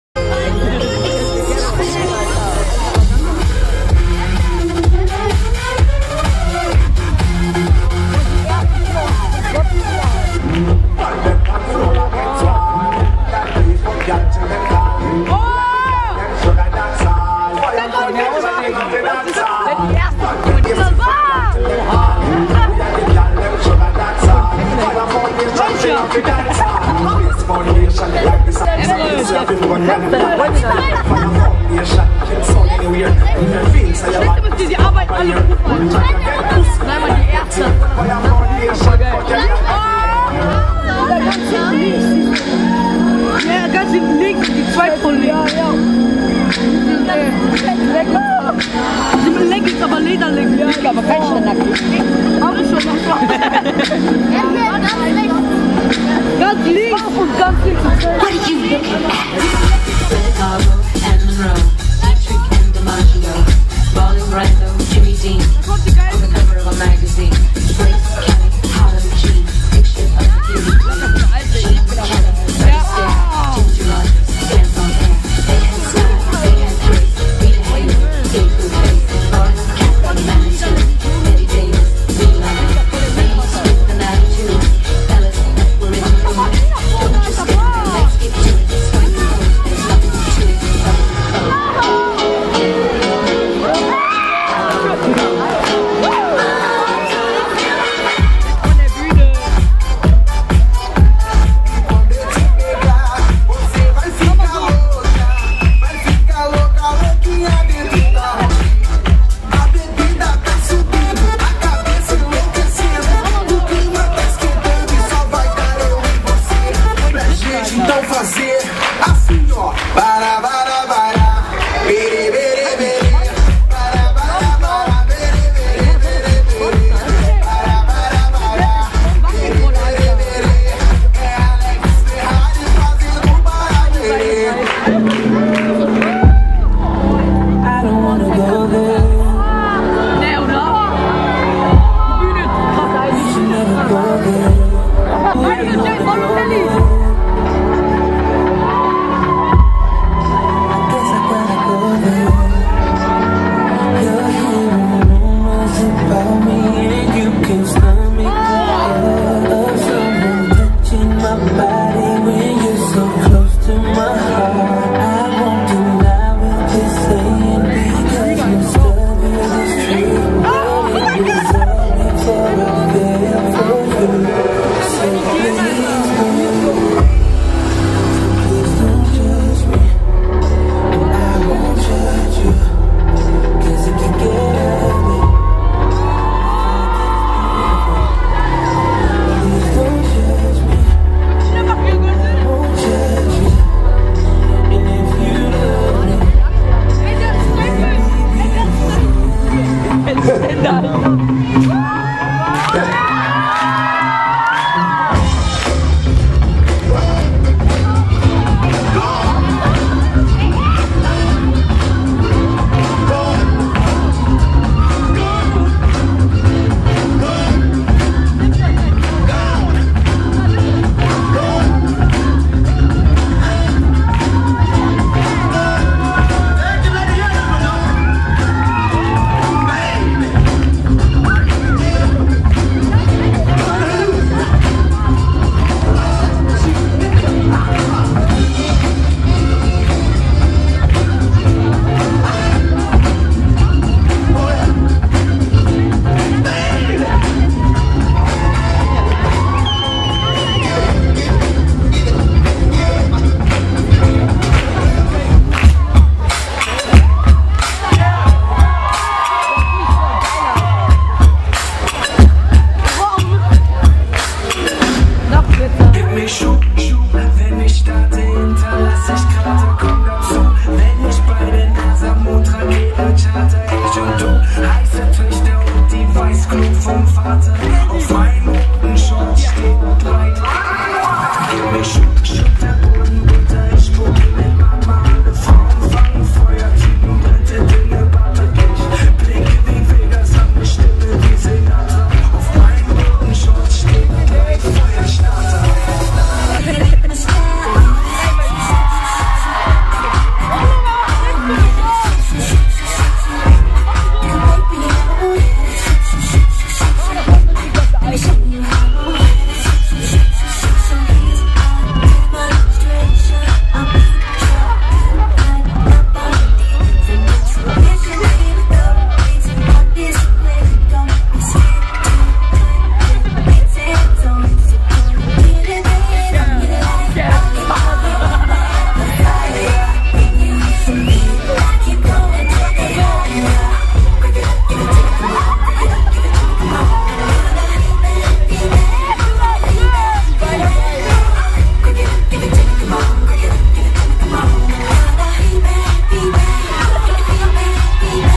Silverfox Crew 2013 mine-sports Team 4 Fashionworld Krefeld
mine-sports Team and dancers
for Fashionworld in Krefeld / Neumarkt Bühne
Choreographers:
Mine
Maja
Marie
Mari
Michelle
Leon
Chan
Dogukan